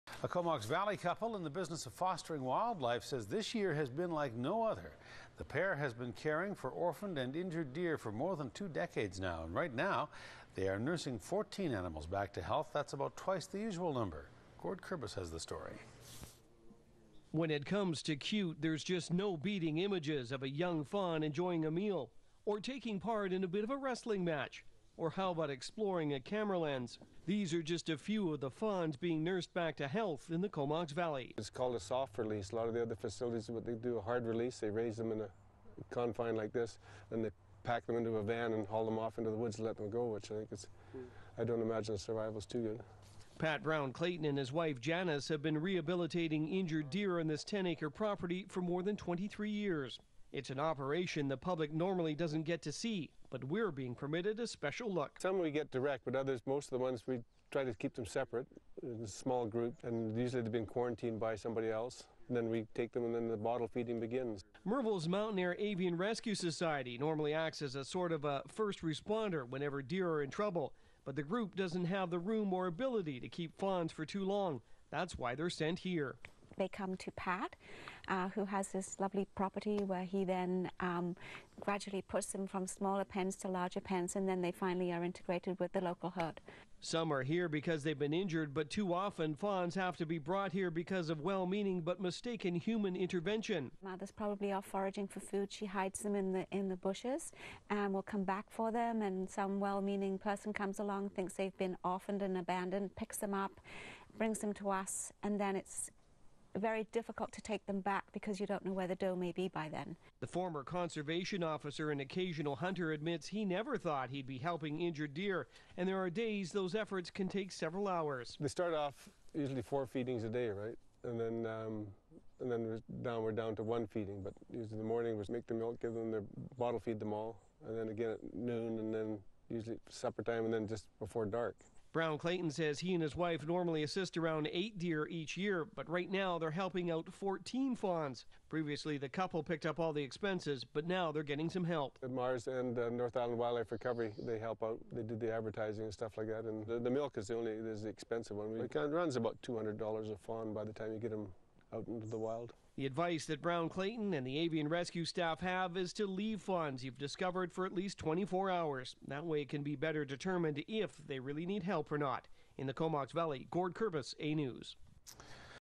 Fostering Deer
A Comox Valley couple, in the business of fostering wildlife, says this year has been like no other.

The pair has been caring for orphaned and injured deer for more than two decades.

Right now they are nursing 14 animals back to health, which is almost twice as many as usual.